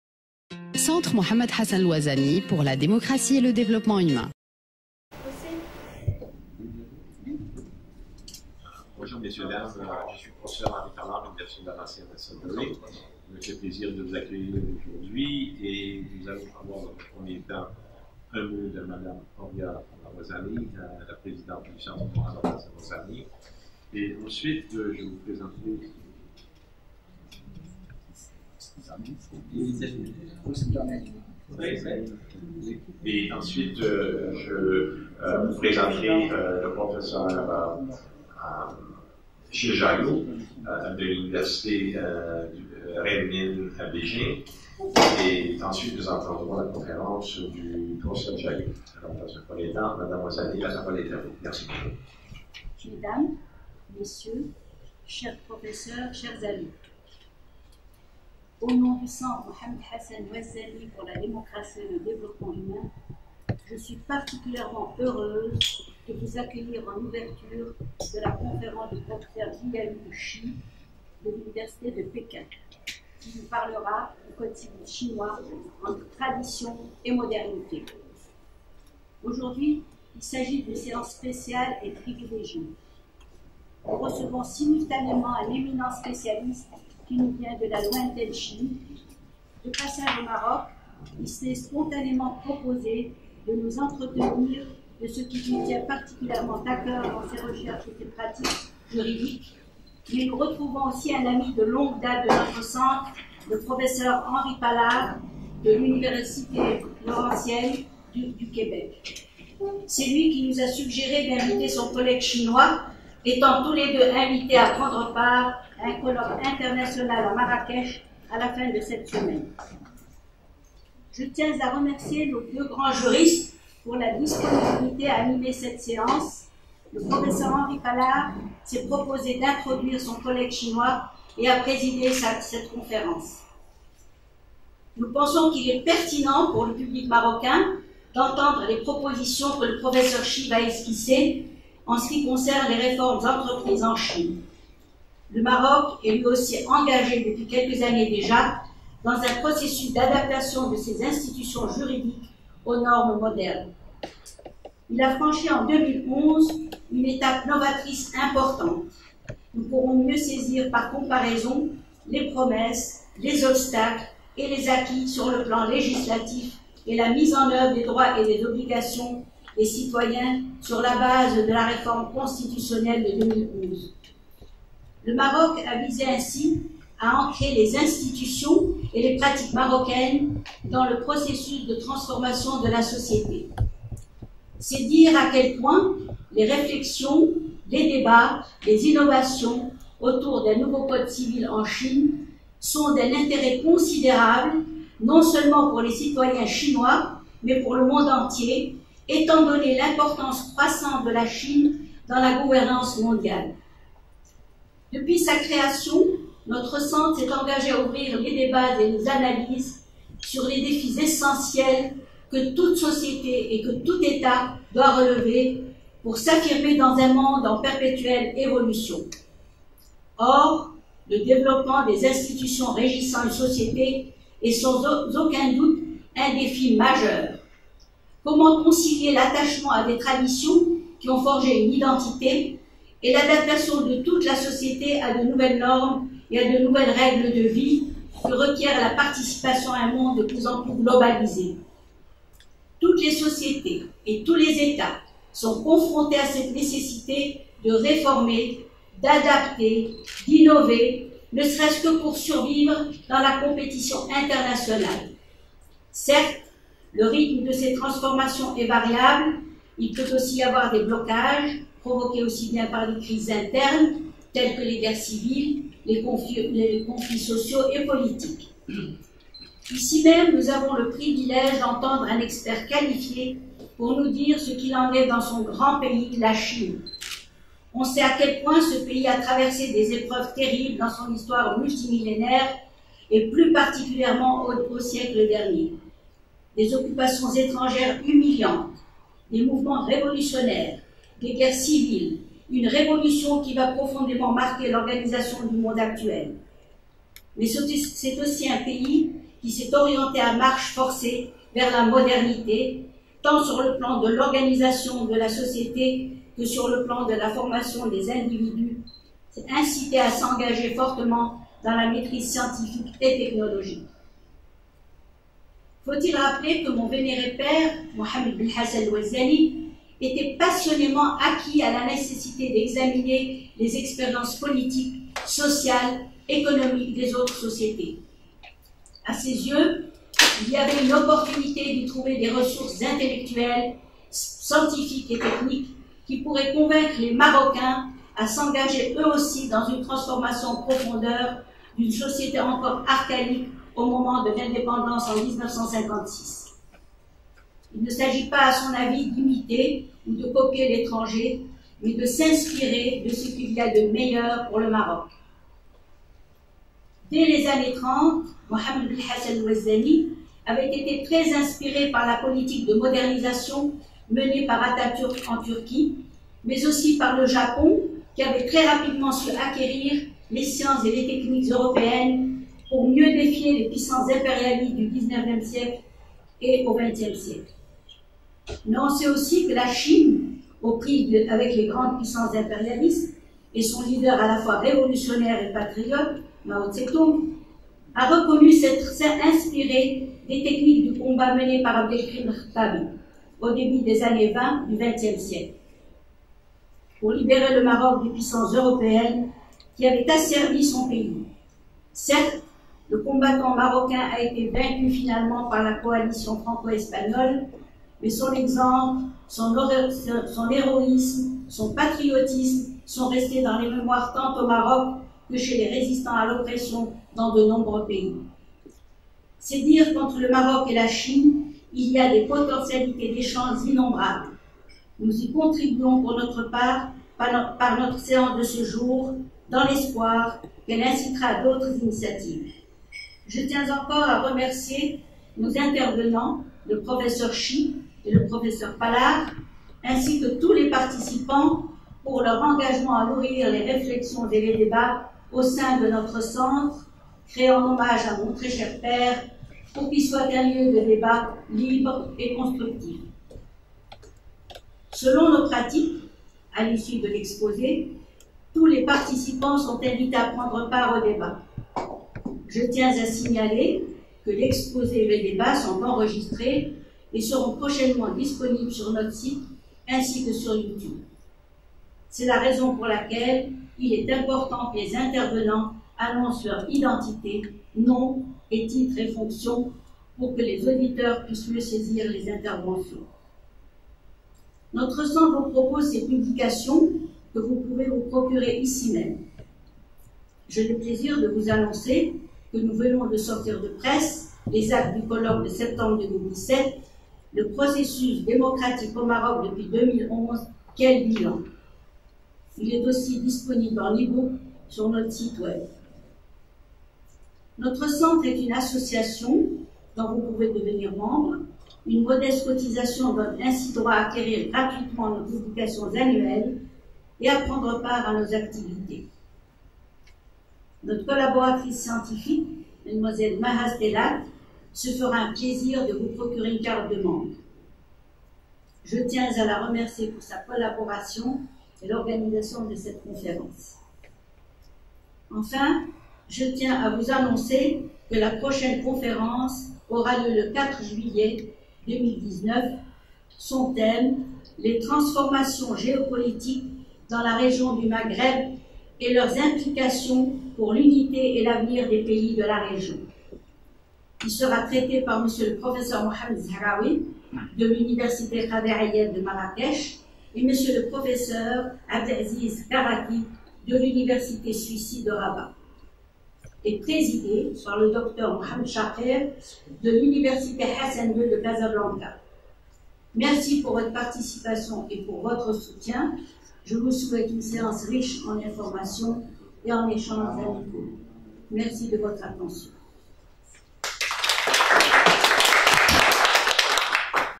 Mot d'accueil de la présidente Dr Houria Ouazzani
Pour sa 5ème conférence au titre de l’année 2019, le Centre Mohamed Hassan Ouazzani pour la Démocratie et le Développement Humain a eu le plaisir d’accueillir
Monsieur le Professeur SHI JIAYOU, Docteur en droit de l’Université de Paris I (Panthéon-Sorbonne ) et Professeur agrégé à l’Université Renmin de Chine.
le Mercredi 12 Juin 2019 à 16 h pour donner une conférence sous le thème:
“ La rédaction du Code civil chinois: entre la tradition et la modernisation”
La conférence a été présidée et modérée par Monsieur le Professeur HENRI PALLARD, Professeur et chercheur à l’Université Laurentienne (Sudbury, Canada).
Nous vous remercions vivement pour votre présence et pour votre participation au débat qui a suivi.
Avec nos meilleures salutations.